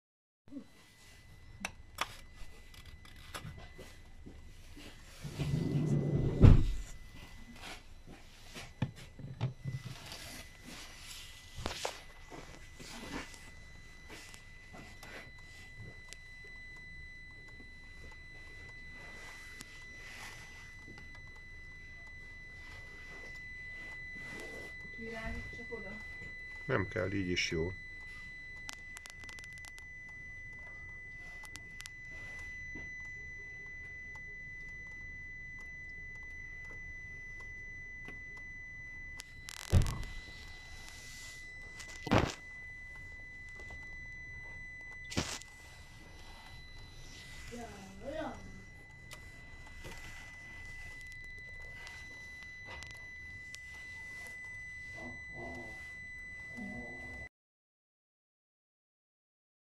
Vezérműszíj Toyota Proace City Verso | Benzin 1.2 | 35850 km | 14 hónapos | 2560 km/hó
Vezérműszíj állapota jó vagy nem jó?!?